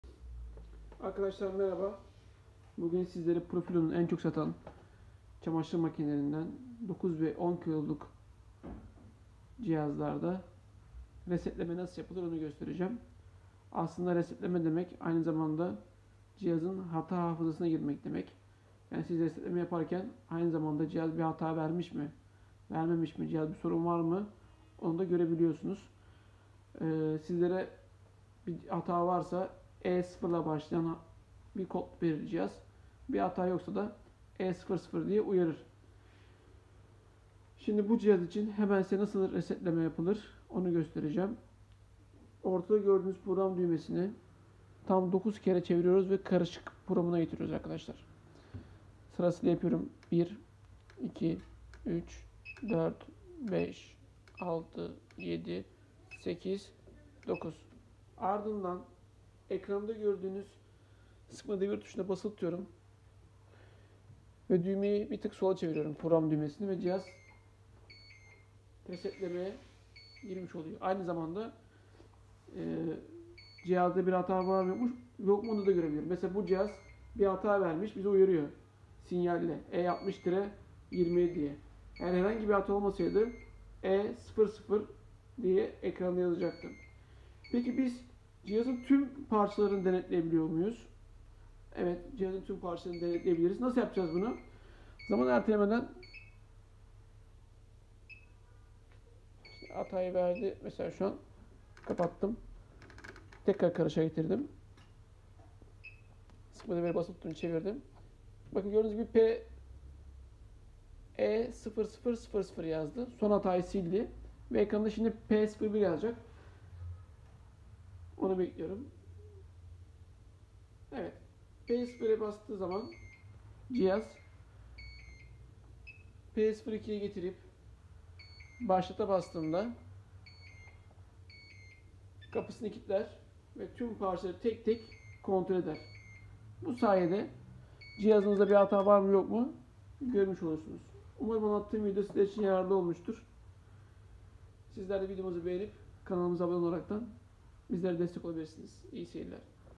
Profilo çamaşır makinesi nasıl resetleme yapılır? CGA242X1TR CGA142X1TR 1200 dv 9 ve 10 kg
Arkadaşlar bu videomuzda sizlere Profilo çamaşır makinesinde resetleme nasıl yapılır. Son hata hafızası nasıl okunur yada cihazda bir ariza var mı nasıl kontrol edilir onu gösterdim. Bu konuyla ilgili soru ve görüşlerinizi yorumlar kısmında belirtirseniz sizlere yardımcı olabilirim.